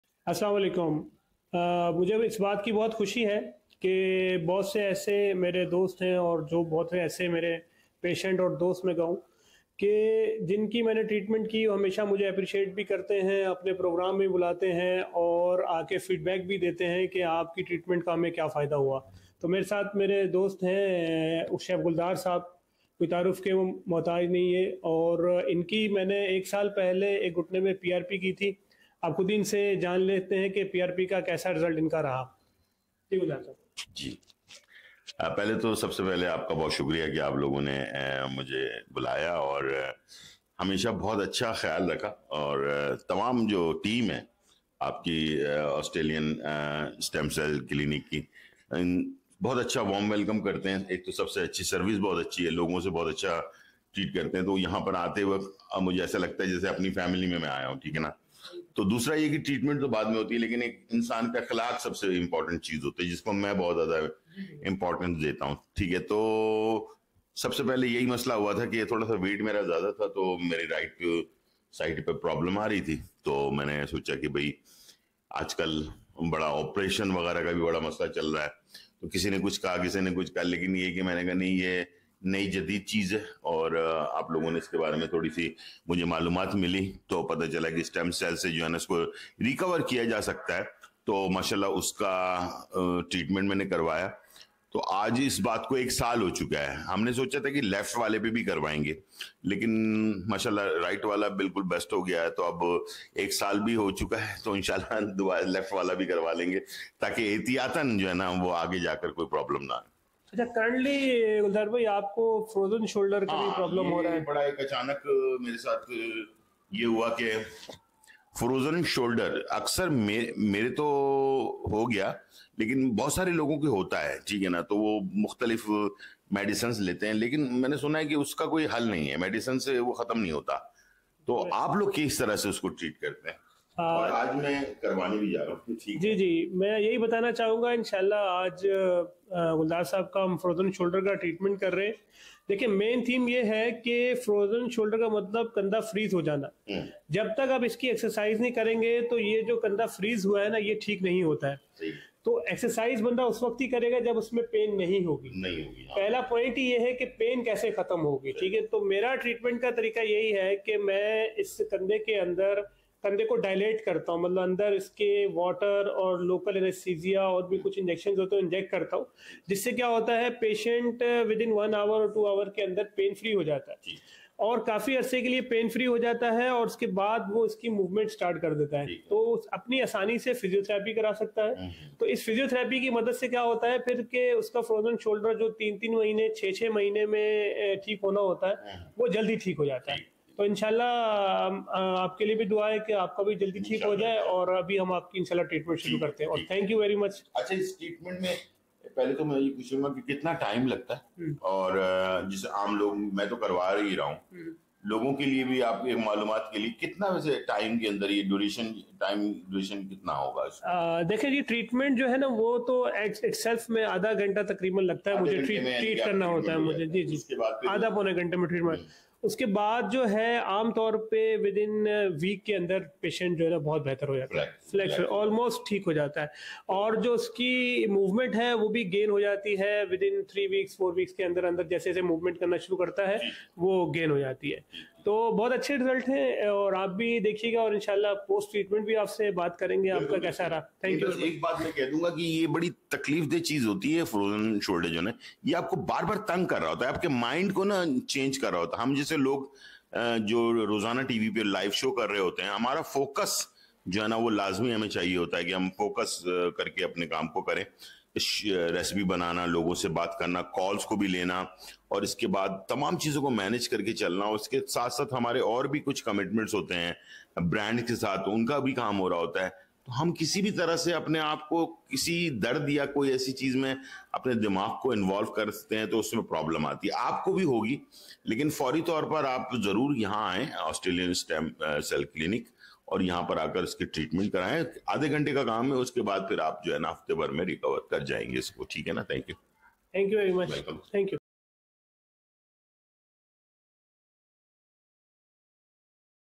Review From Chef Gulzar about his treatment for knee pain | Dr Latif
Find us at:
📍 D-102/2 Sure Cell Australian Stem Cell Clinic
Clifton Block 2, Ziauddin Hospital Road, Karachi.
Reclaim your comfort and get back to the activities you love! 🚀